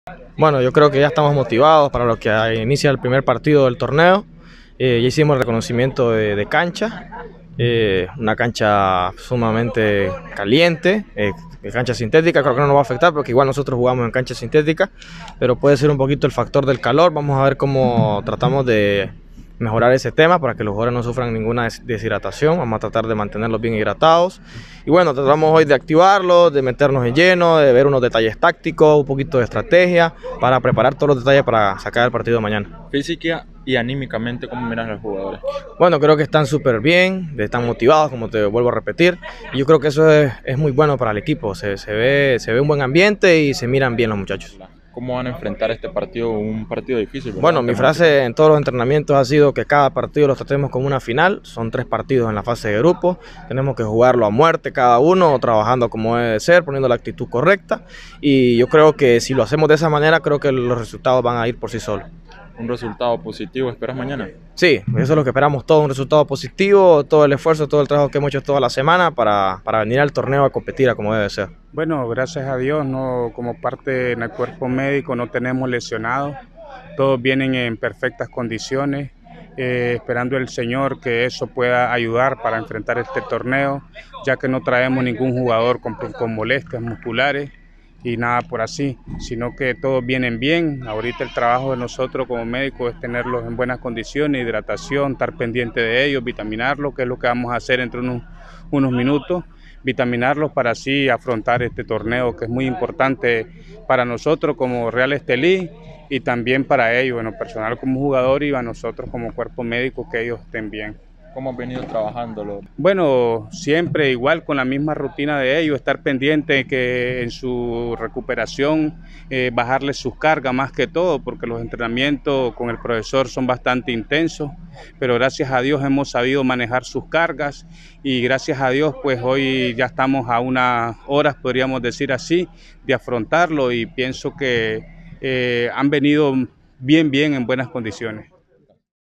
Interclubes U17 - Reconocimiento de cancha
El DT Holver Flores y el Dr. Iván González comentan sobre las proyecciones del plantel tras realizar la primera sesión de entrenamiento en Choluteca.